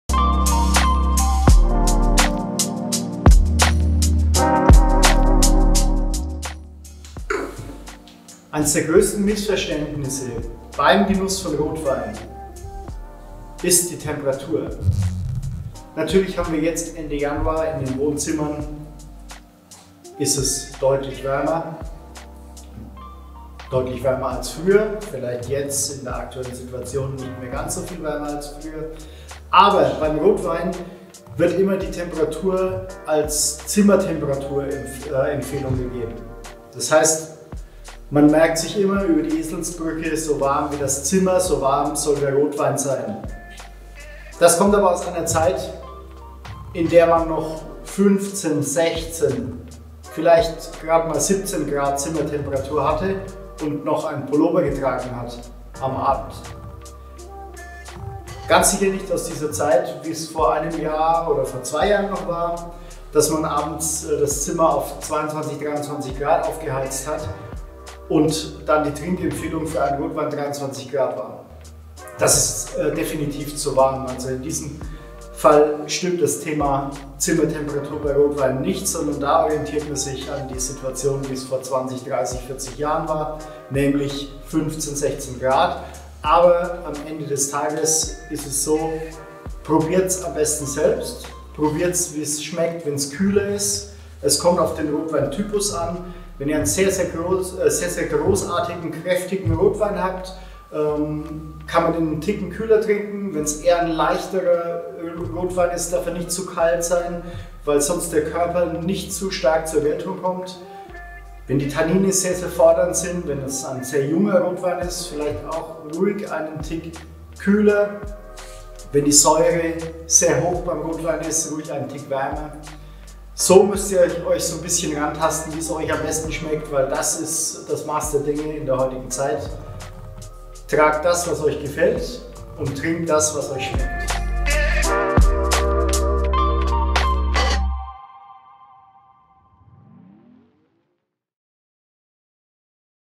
Stahl x Mey & Edlich – Zimmertemperatur bei Rotwein.
Weinkennern kennen Christian Stahl als den Winzer, der in Deutschland die Weinszene aufmischt. Dem Männermodeunternehmen Mey & Edlich hat er das Mysterium Zimmertemperatur bei Rotwein erklärt. In diesem Video erfährst du, woher der Begriff Zimmertemperatur kommt und bei welcher Temperatur Du den Rotwein perfekt genießen kannst.